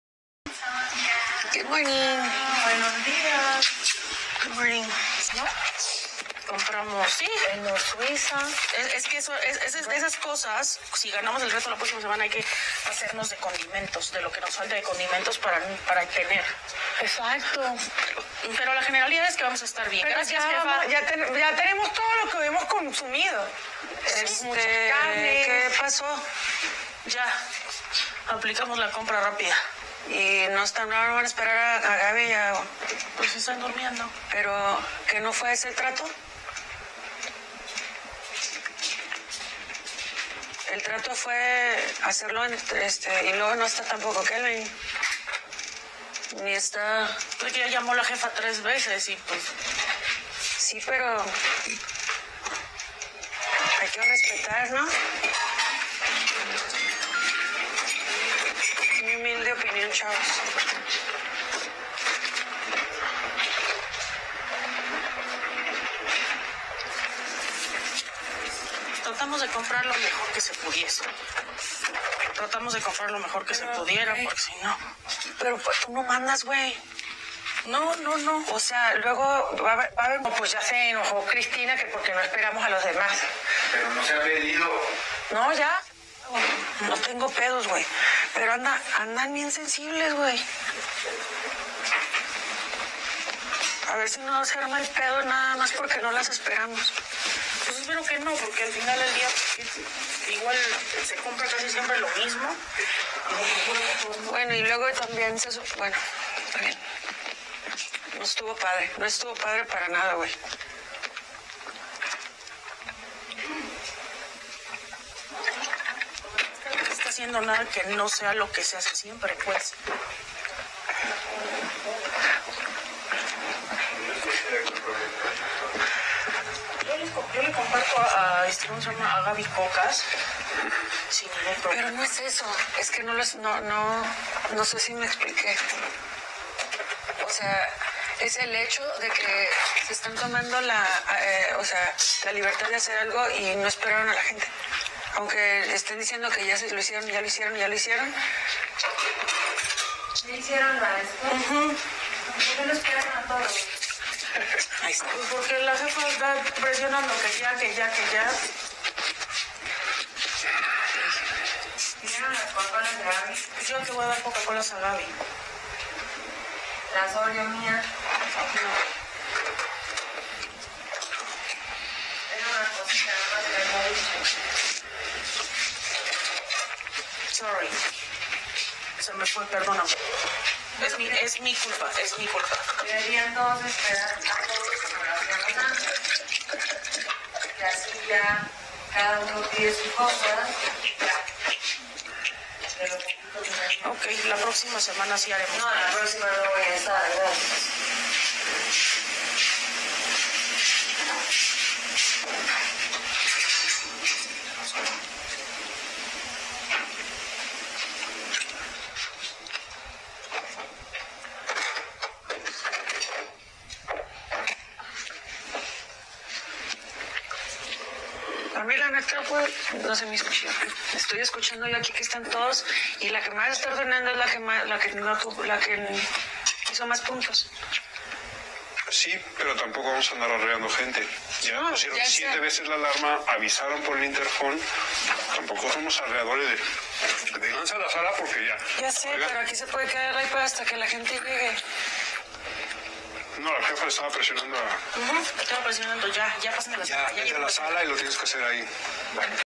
El pleito por las compras del super, incluyendo los lamentos de Verónica
Pudimos rescatar como inicio el pleito, seguimos buscando en la memoria de la computadora el archivo completo.